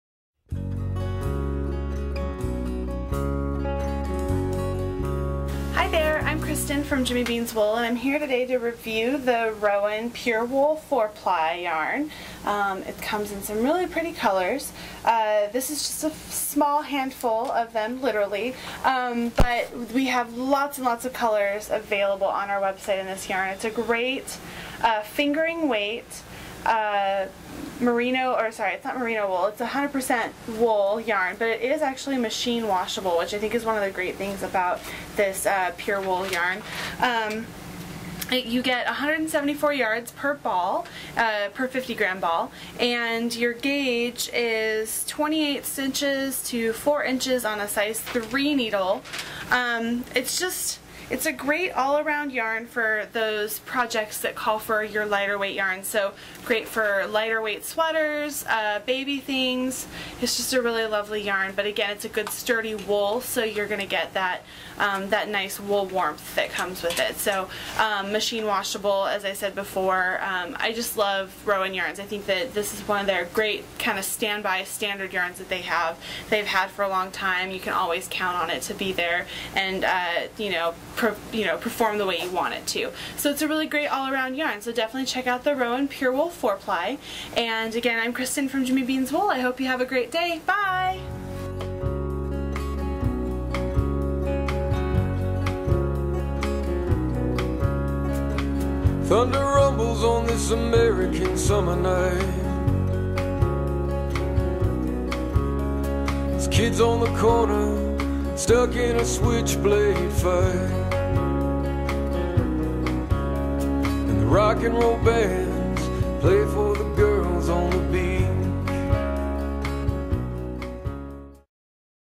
Rowan Pure Wool 4 Ply Yarn Review
Kristen from Jimmy Beans Wool reviews Rowan Pure Wool 4 Ply Yarn.  Jimmy Beans Wool is your LYS... online!